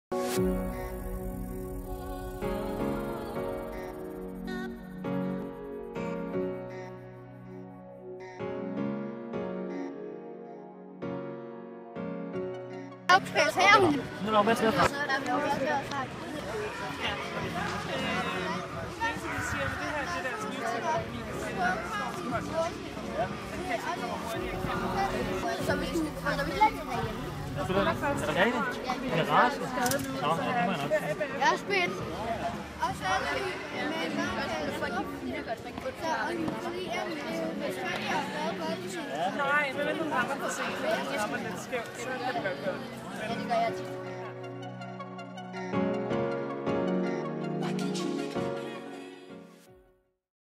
25 anbragte børn mødte Caroline Wozniacki hos KB Tennis på Frederiksberg
Den internationale stjernespiller var i Danmark for at give anbragte børn og unge inspiration til et aktivt fritidsliv.

Formålet med tennis-eventen var at motivere de unge deltagere til mere fysisk aktivitet i hverdagen. Caroline selv kunne ikke spille på grund af en alvorlig skade i foden, men børnene fik alligevel prøvet kræfter med den populære ketchersport.

Det var Børnehjælpsagen og Carolines danske sponsor e-Boks, der stod bag arrangementet d. 25. oktober 2016.